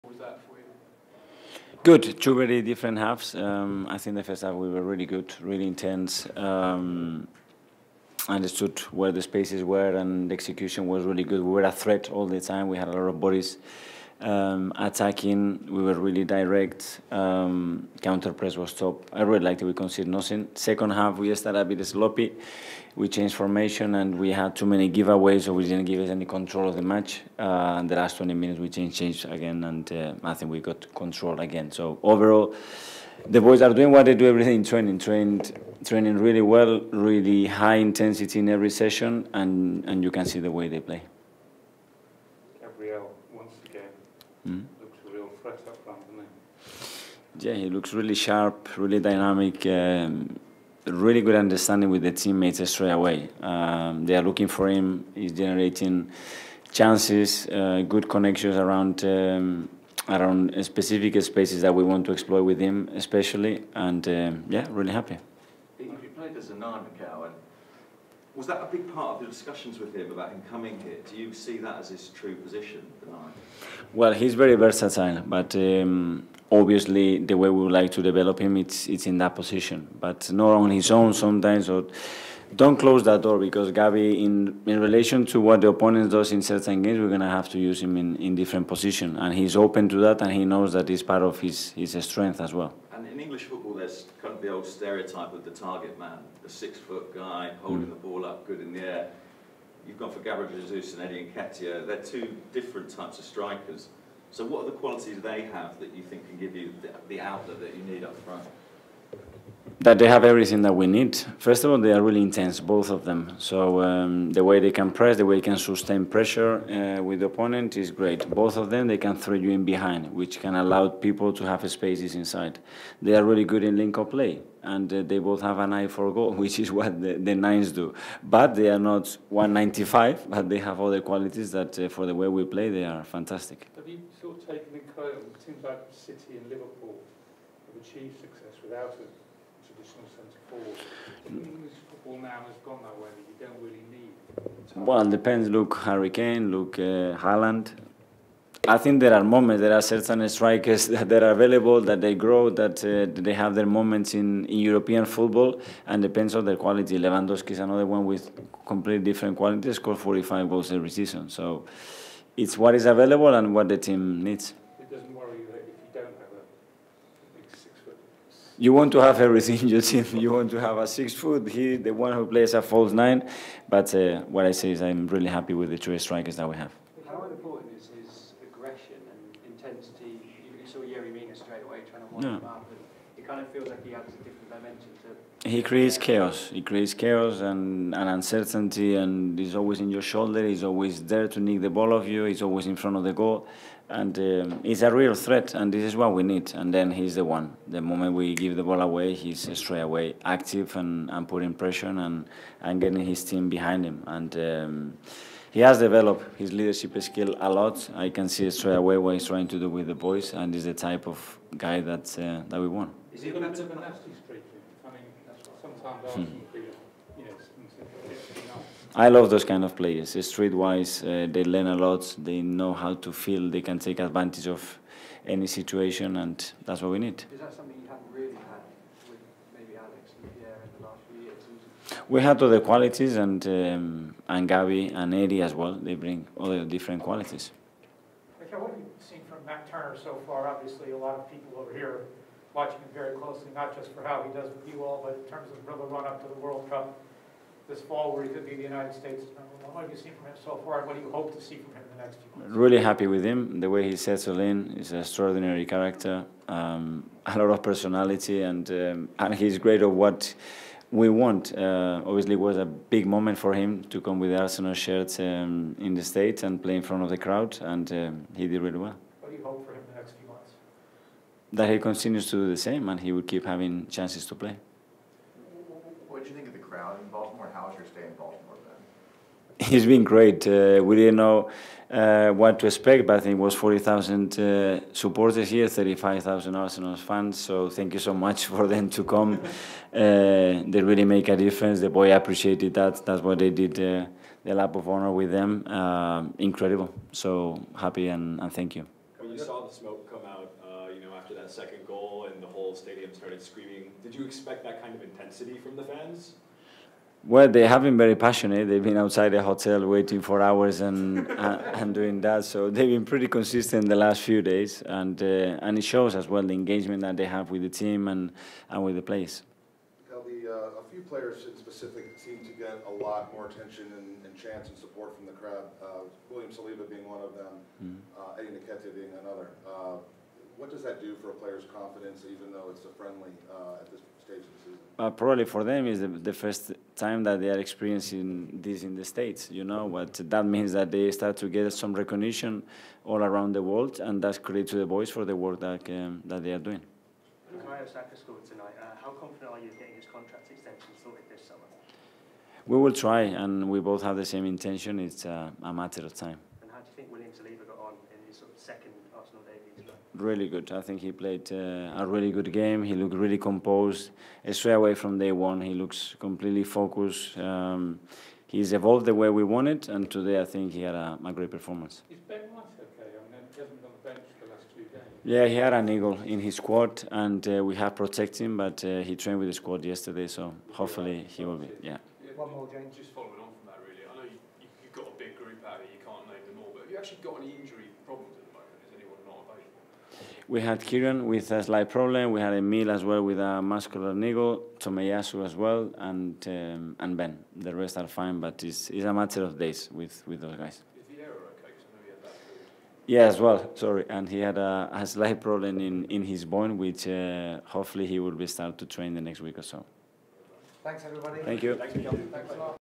"It's only a matter of time for Saka's NEW contract!" | Mikel Arteta | Everton 0-2 Arsenal
Arsenal manager Mikel Arteta says it is only a matter of time before Bukayo Saka signs a new deal at the club.

-TIMESTAMPS-

00:00 - Intro

00:05 - Really good. Two different halves, but we were a threat all the time.

00:45 - Gabriel Jesus looks really sharp. He has a really good understanding with the team straight away.

01:15 - Jesus is very versatile, but we want to develop him as a number nine.

01:50 - Nketiah and Jesus have everything we need. They are both really intense.

02:25 - Look at Kane and Haaland. They have their moments in football. Certain striker develop and grow. Every striker has different qualities.

02:50 - You want to have everything, but I'm really happy with the strikers we have.

03:05 - Jesus creates chaos and uncertainty. He's a real threat and he's the one we wanted.

03:40 - I love these types of players, they're street wise and can take advantage of any situation.

03:55 - We have all the qualities, but Eddie and Gaby offer us different qualities.

04:15 - I'm really happy with how Turner has settled in. He's an extraordinary character. It was a big moment for him.

04:35 - If he keeps doing what he's doing then he'll have chances to play.

04:45 - Our stay in Baltimore has been great. Having 35,000 Arsenal fans in the stadium was incredible.

05:10 - The fans have been very passionate, standing outside our hotel for hours!

05:35 - For some of the players, it's probably the first time they've experienced this.

05:55 - Saka and I have the same intentions and it's only a matter of time.

06:00 - Saliba played a really good game, he's evolved the way we wanted.

06:15 - Ben White had a niggle in his quad but will hopefully be training tomorrow.

06:30 - We have Tierney with a problem, Smith-Rowe with a muscular niggle, Tomiyasu as well and White. But it's a matter of time.